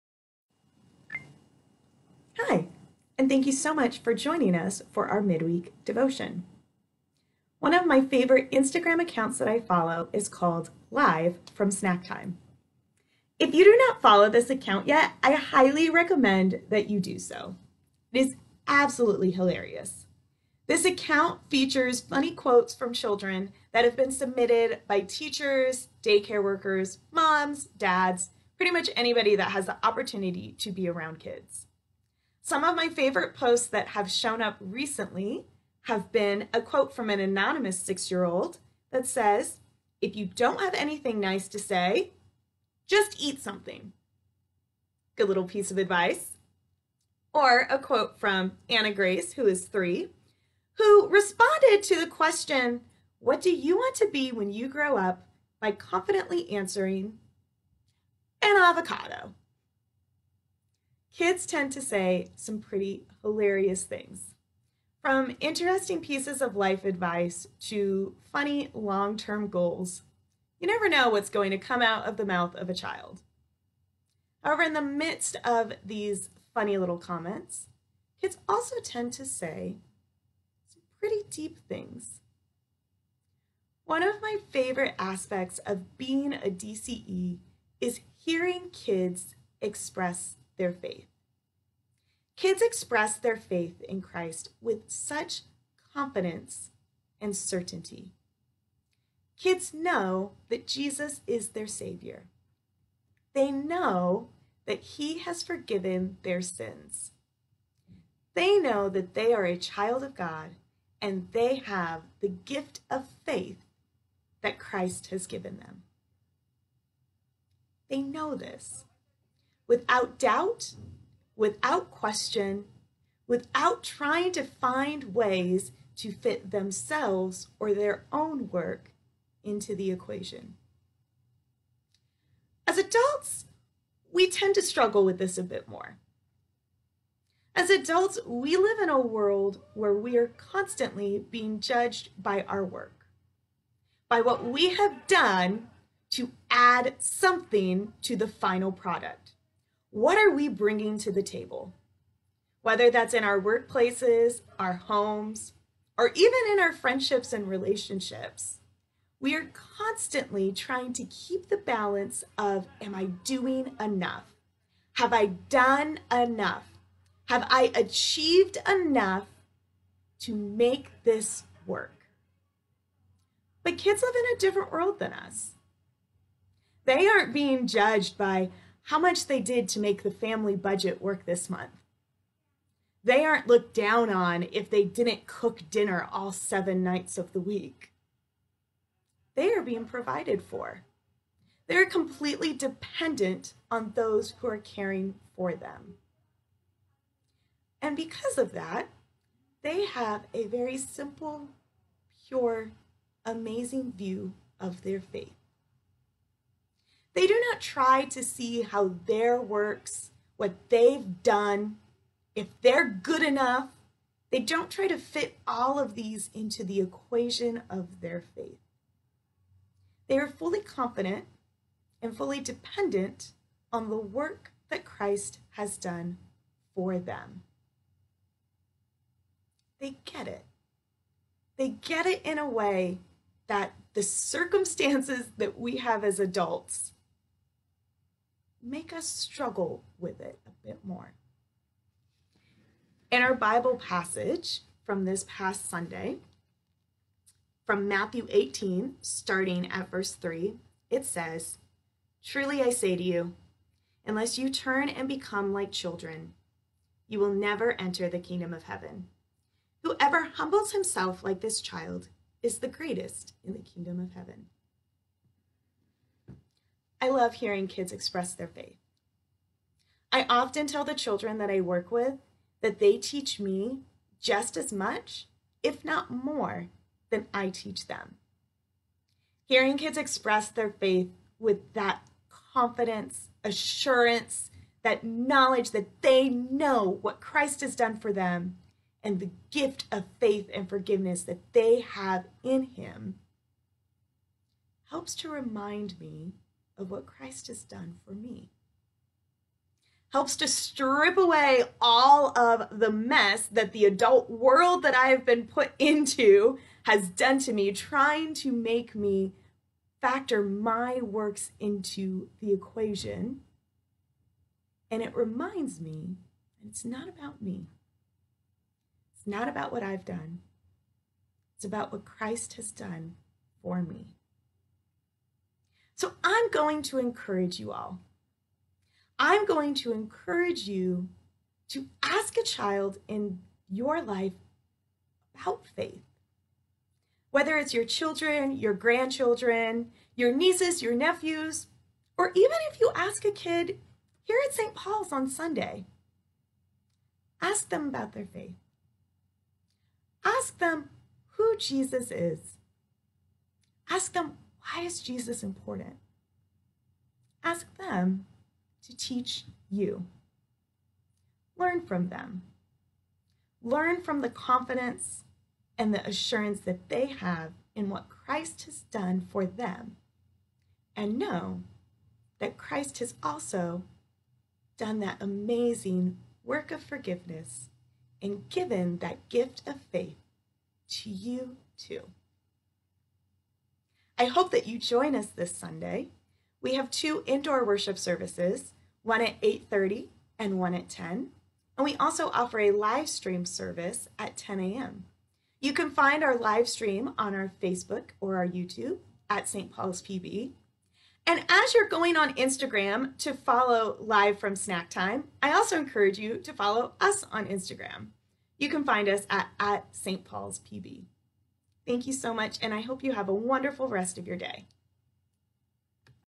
Weekly Devotion: Childlike Faith
Laura Bredehoft, DCE explores faith in this week's devotion.